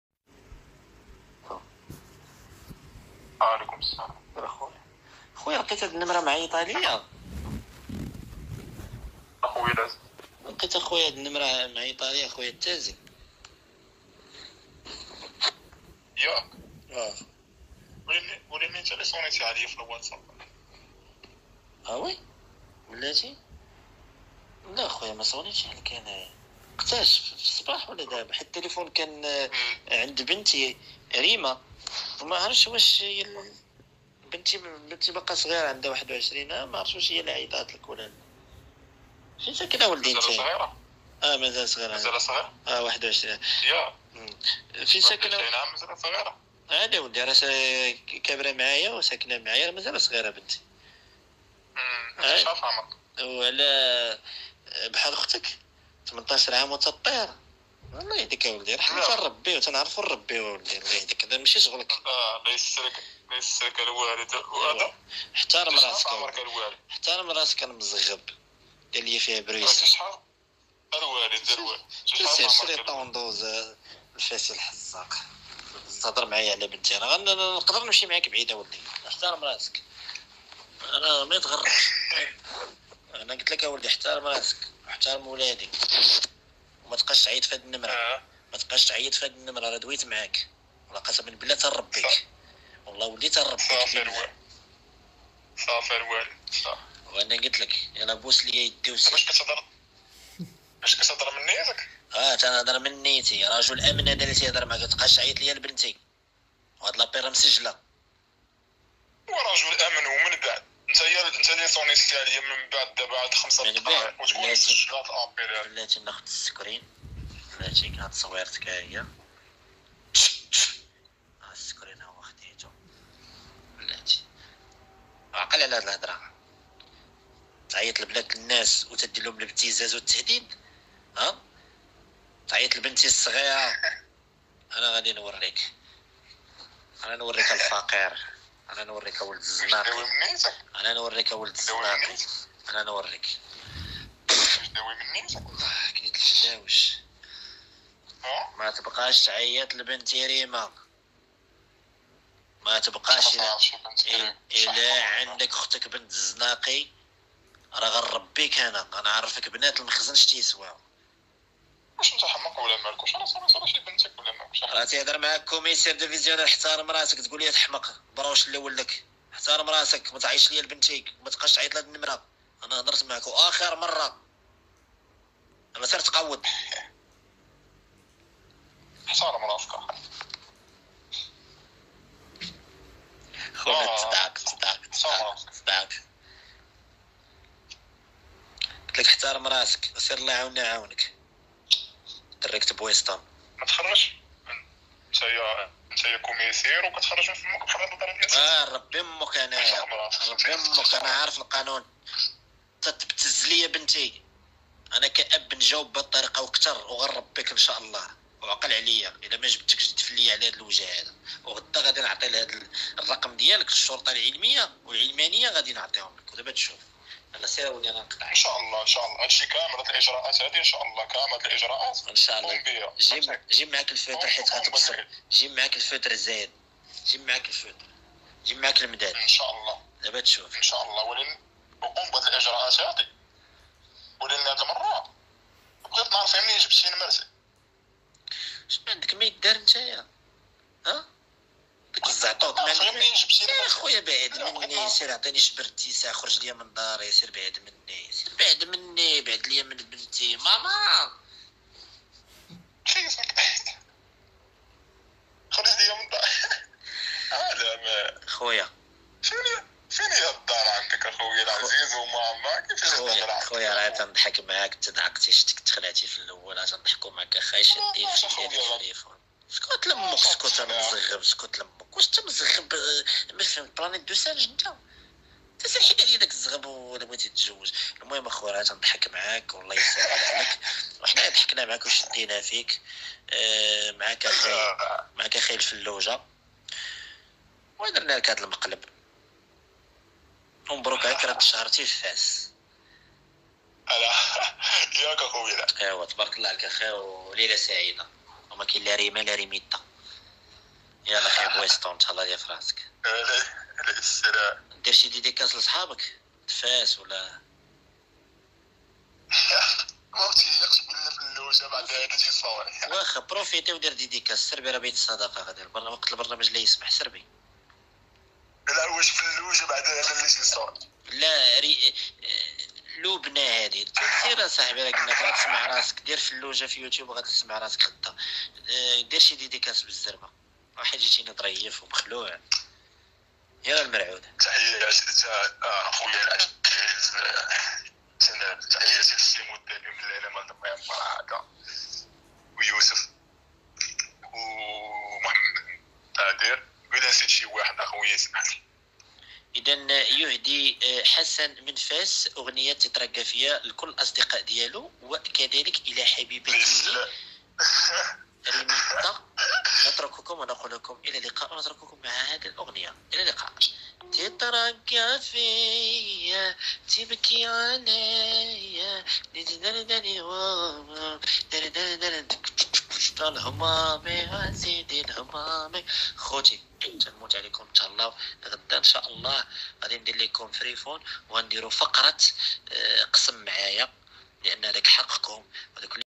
Falouja Vs Fassi فلوجة داير فيها كوميسير ومطلعها على الفاسي المسكين
شاركوا في قناتي من خلال الرابط اسفله. فعّل زر التنبيهات ( 🔔 ) ليصلك كل جديد